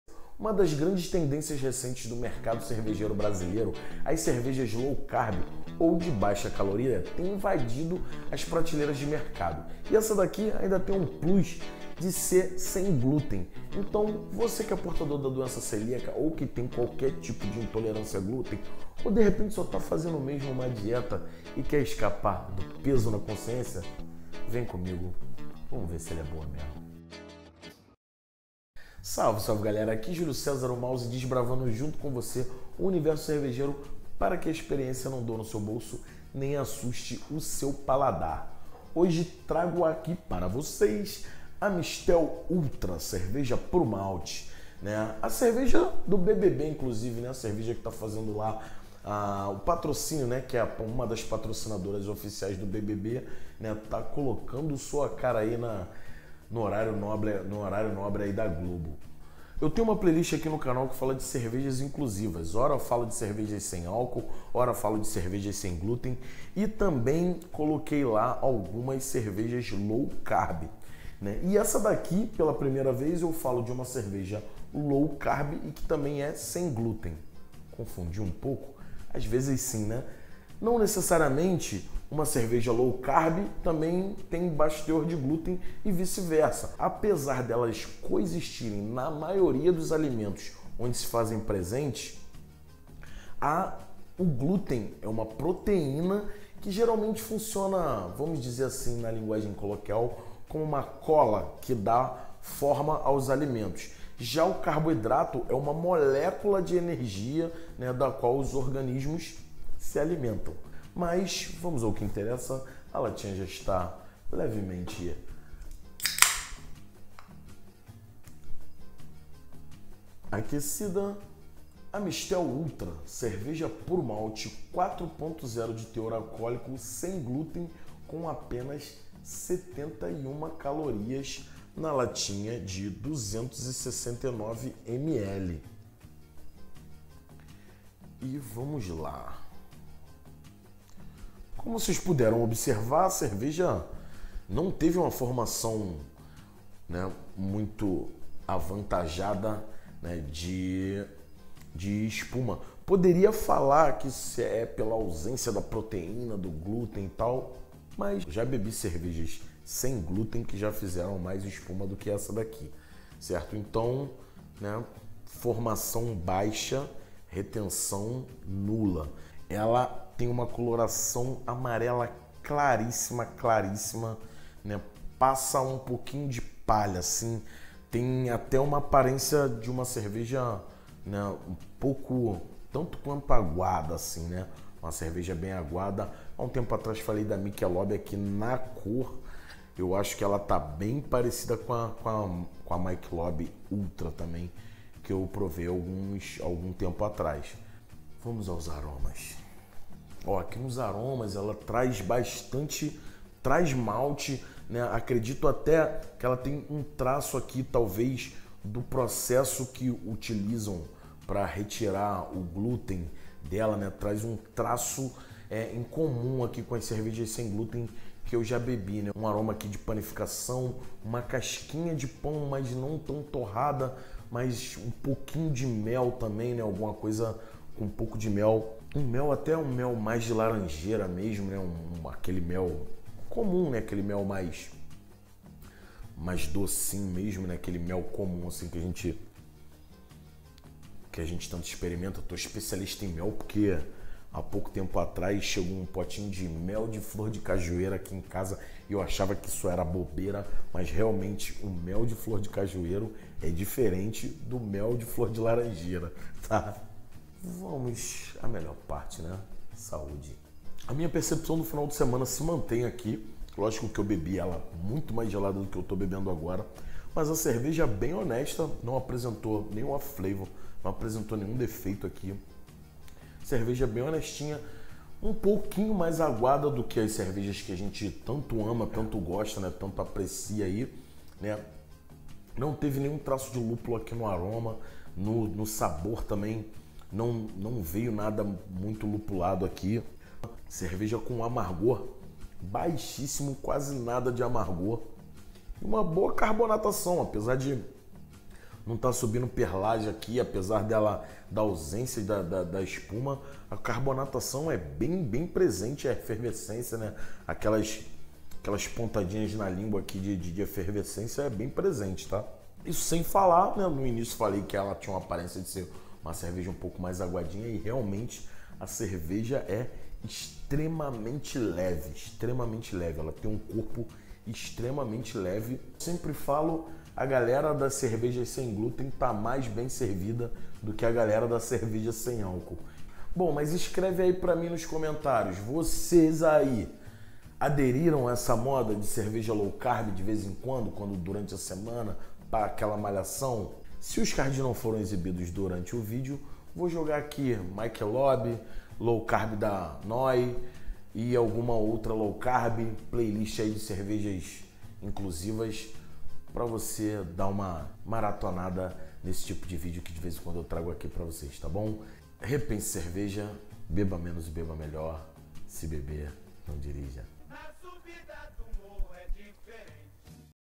AMSTEL ULTRA - CERVEJA LOW CARB SEM GLUTEN
Pode parecer óbvio uma cerveja low carb tambem ser sem glúten, mas não é nem um pouco. A ausência ou baixo teor de um não garante a mesma coisa no outro. A Amstel se mostrou umas das melhores opções, tanto do mercado low carb quanto para o mercado de cervejas sem glúten. Na minha humilde opinião. Uma cerveja leve em todos os sentidos com apenas 71 calorias por latinha de 269ml e 4% de álcool .

00:00 - Uma ceveja low carb já invadiu as prateleiras dos mercados e dos realities.
00:59 - Outras cervejas inclusivas já foram abordadas e possuem playlist própria low carb e sem glutén ao mesmo tempo é a primeira.
01:51 - O serviço na latinha com temperatura maior do que normalmente iremos beber para facilitar a volatilidade dos aromas.
02:22 - Já na aparência todo o jeito de cerveja de massa que vai apresentar as características que comprovem isso.
03:17 - O serviço em temperatura mais alta me facilitou muito na hora de avaliar os aromas afinal, é uma cerveja pra ter pouco aroma mesmo e a temperatura potencializou muito facilitando muito minha vida.
04:17 - Finalmente a melhor para, ao menos para mim. Mas acho que não ficaria tão interessante sem as partes anteriores. Saúde!
05:26 - Um conclusão que sempre tiro quando bebo cerva sem glúten, fala comigo!